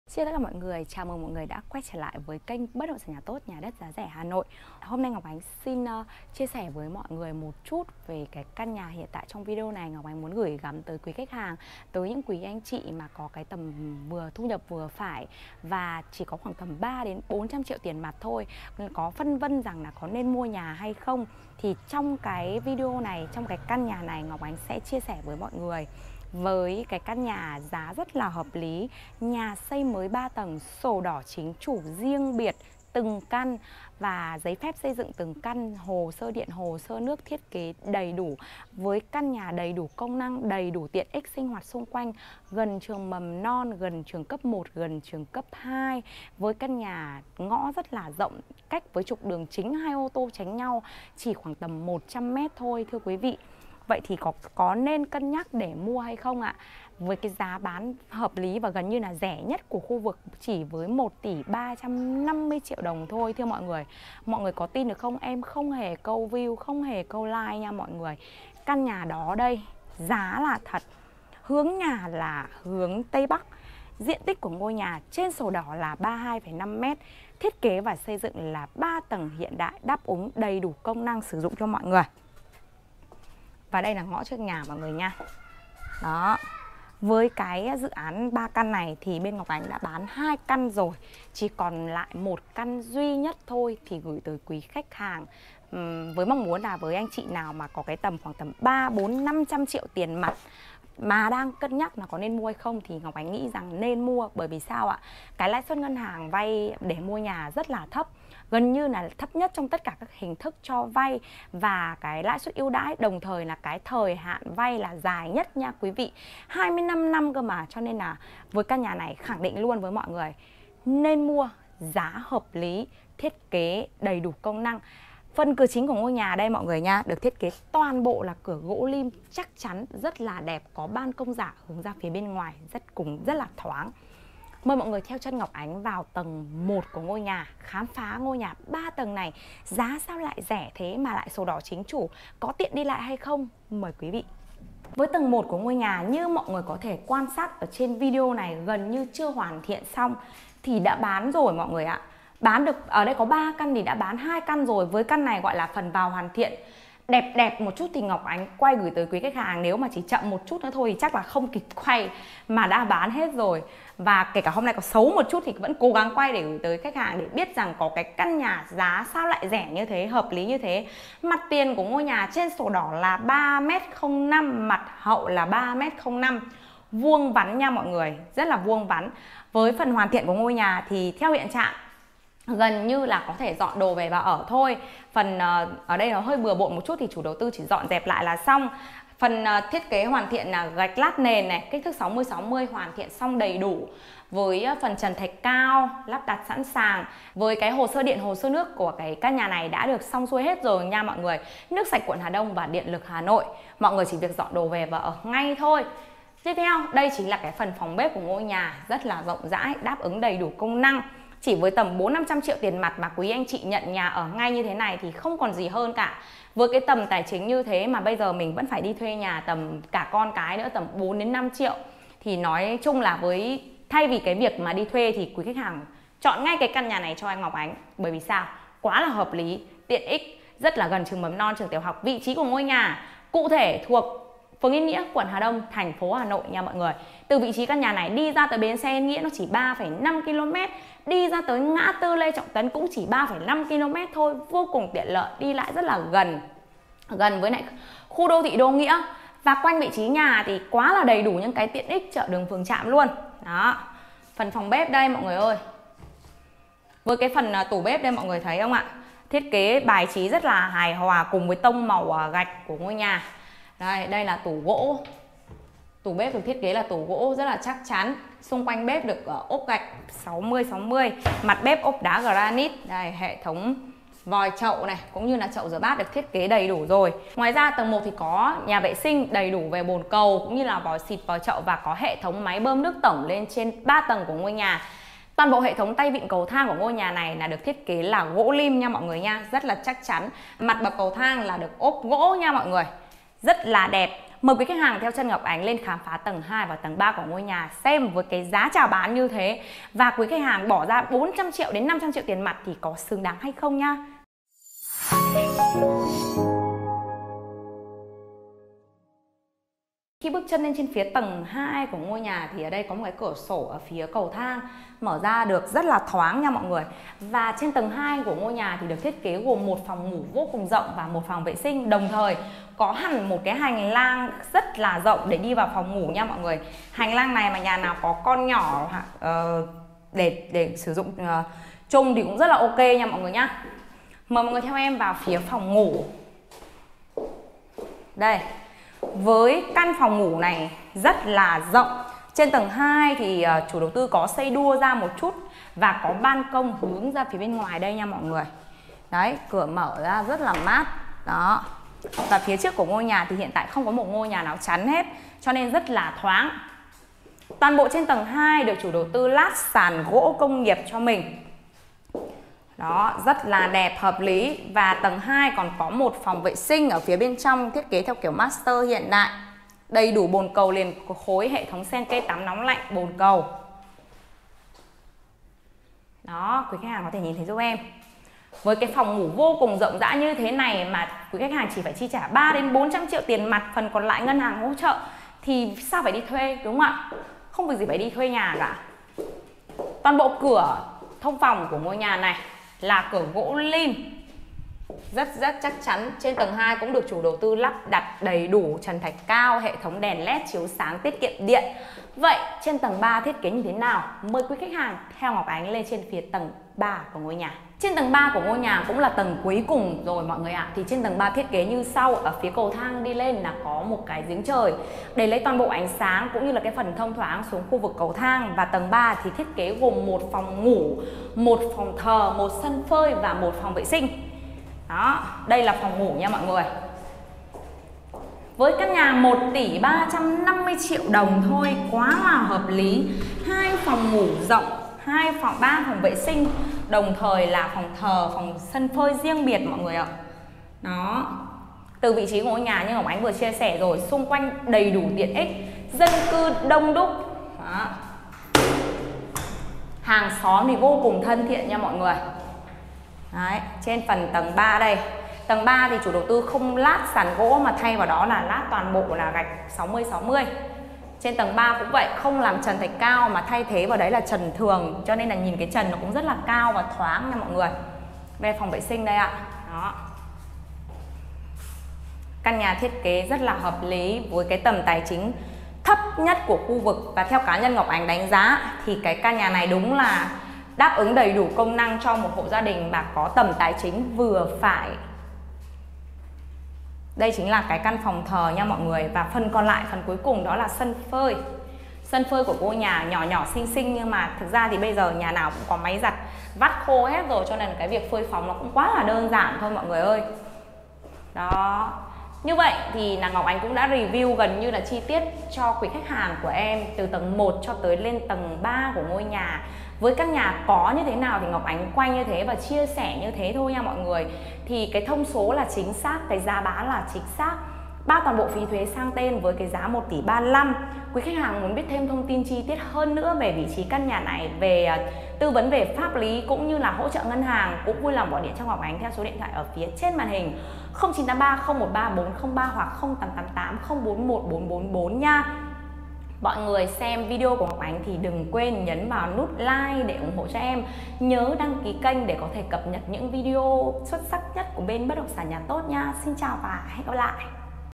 33m2 x 3 Tầng, Giá Rẻ Nhất Quận Hà Đông, Hà Nội | nhà TỐT
33m2 x 3 Tầng, Giá Rẻ Nhất Quận Hà Đông, Hà Nội | nhà TỐT
- Địa chỉ: Yên Nghĩa, Hà Đông, Hà Nội
- Diện tích: 33m2
- Hướng nhà: Tây Bắc
- Thiết kế: 3 tầng, 3PN, phòng khách, bếp, sân phơi, 3WC
- Giá bán: 1,35 Tỷ